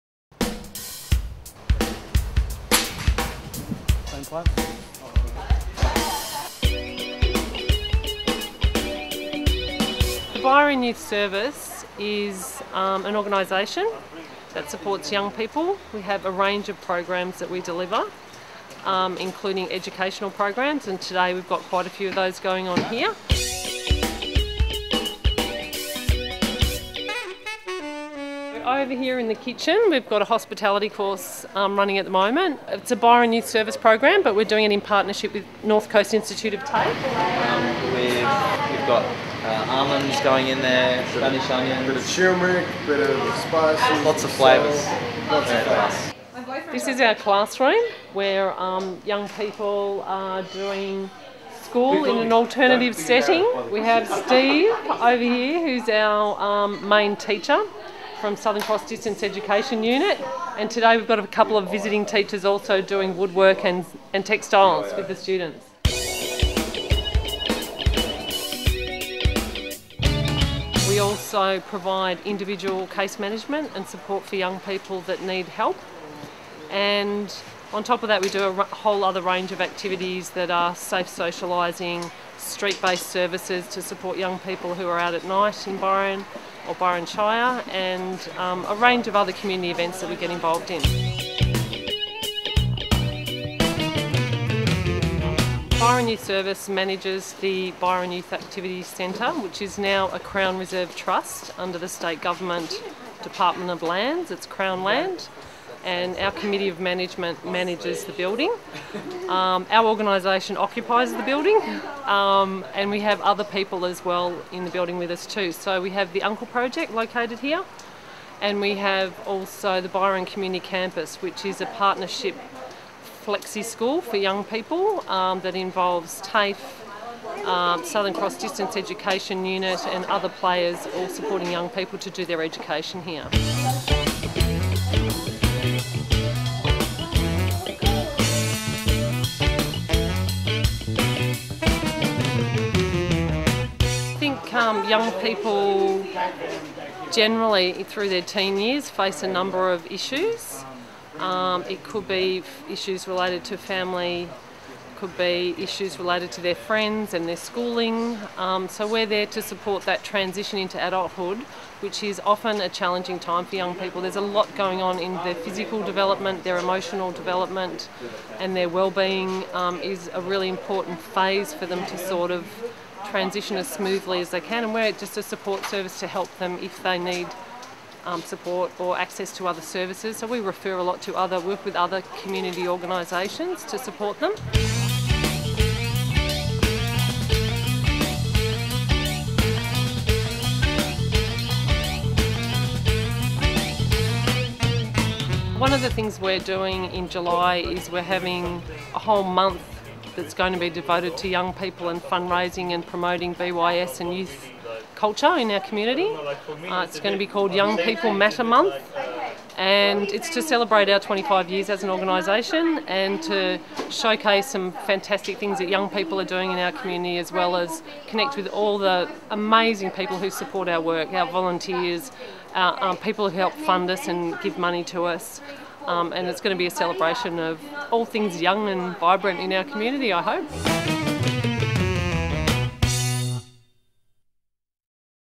Byron Youth Activities Centre
The Byron Youth Activity Centre (the YAC) is managed by Byron Youth Service. For details of activities happening at the YAC, or to hire the venue for youth events, contact Byron Youth Service on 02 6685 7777 or visit their
Filmed & Edited Sharon Shostak, Music by Red Eyed Frogs